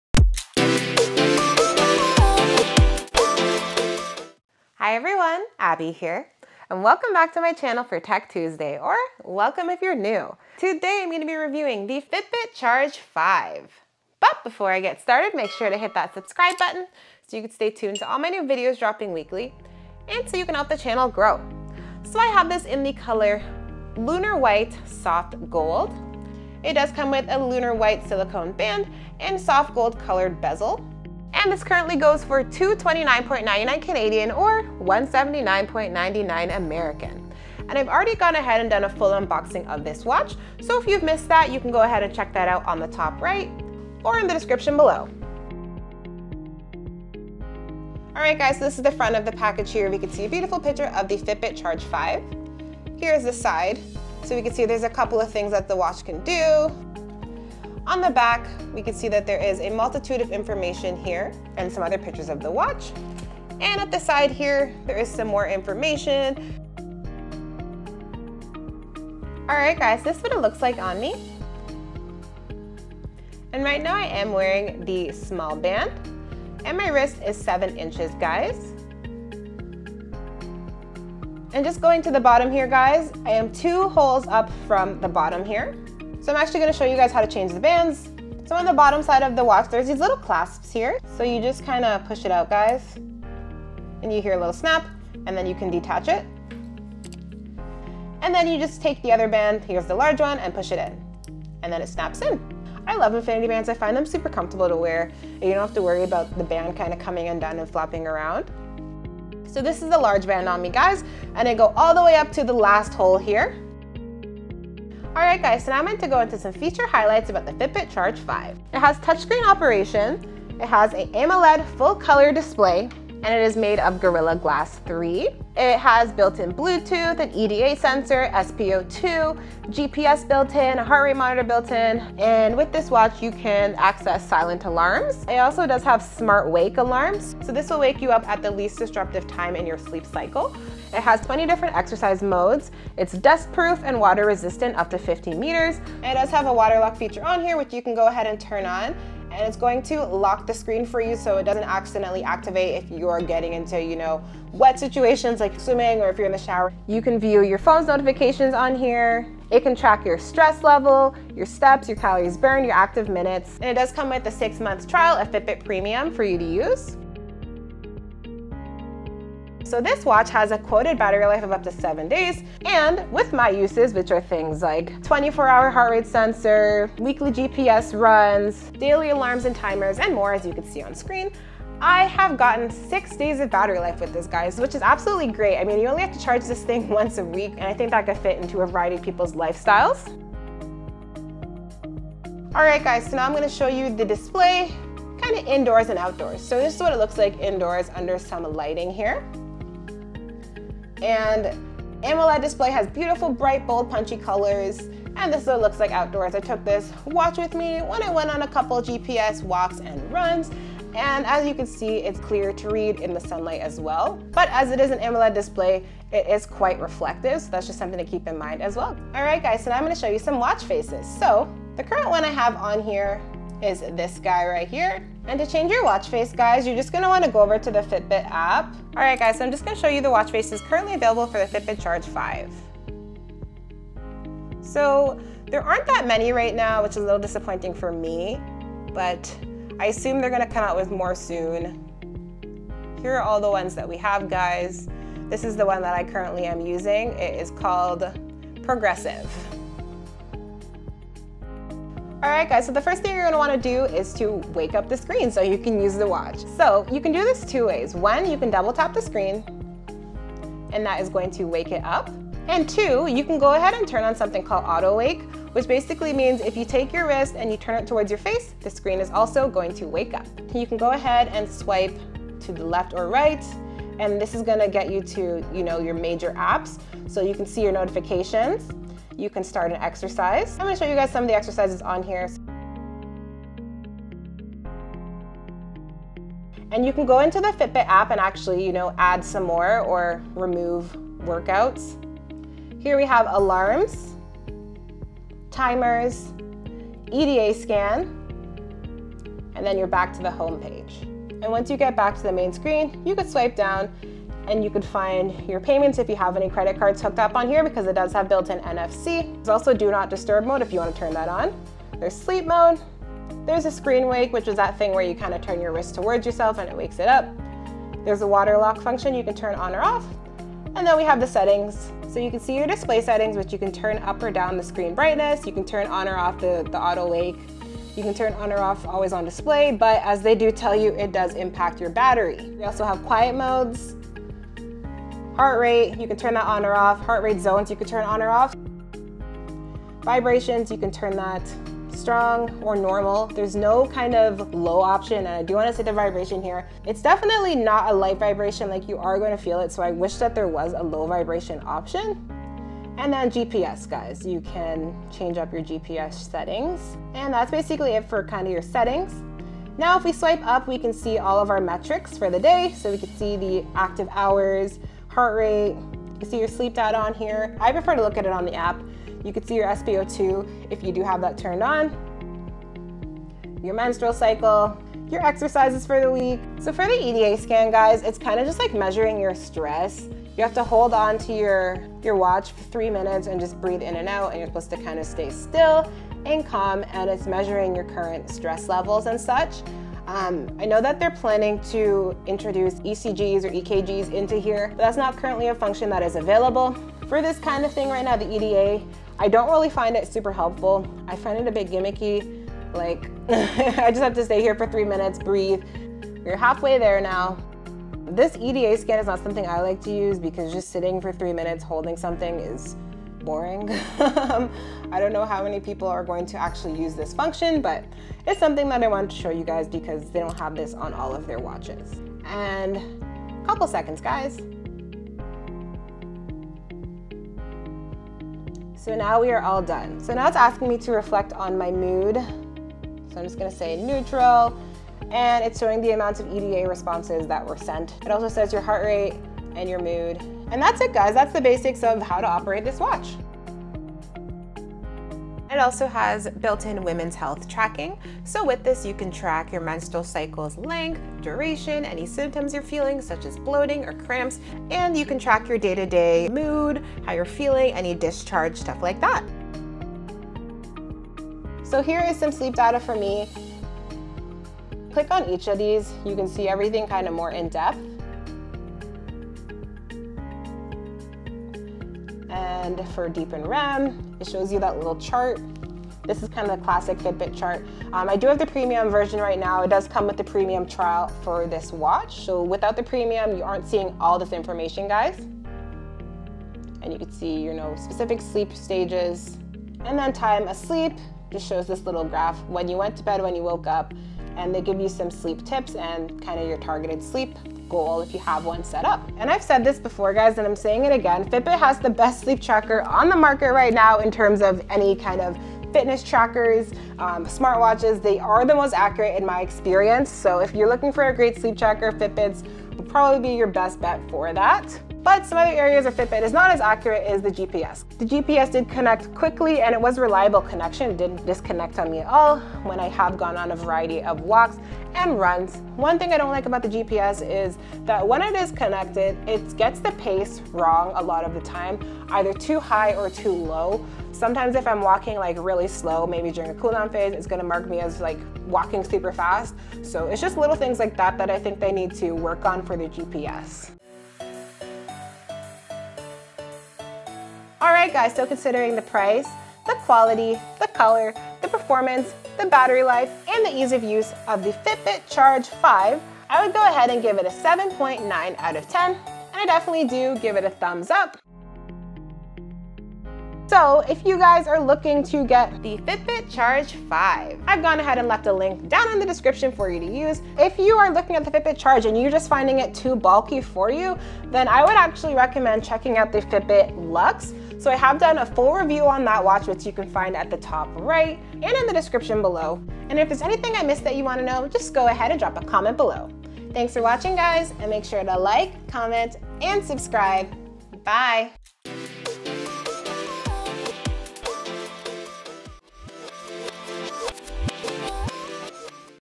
Fitbit Charge 5 Review - Worth It?
This is a review of the new Fitbit Charge 5 Soft Gold Lunar White Smartwatch.

You'll really help my channel AbbyBReviewing grow & allow me to keep making videos for you! :)

Please hit that 👍 button if you found this video helpful :)

Key Points⏰
0:00 Intro
00:23 Colour
00:31 Fitbit Charge 5 Price
00:45 Packaging
01:06 Fitbit Charge 5 Try On Small Band
01:26 How To Change Fitbit Charge 5 Band
01:49 Fitbit Charge 5 Try On Large Band
01:57 Feature Highlights
02:47 Fitbit Charge 5 Battery Life
03:10 Display Indoors & Outdoors
03:39 Fitbit Charge 5 Watch Faces
04:09 How To Use Fitbit Charge 5
06:16 Fitbit Charge 5 EDA Scan
07:23 Women's Health Monitoring
07:39 Sleep Tracking
08:34 Fitbit Charge 5 GPS
09:03 Rating
09:20 Final Thoughts / Outro

Music ♬ from Filmstock music library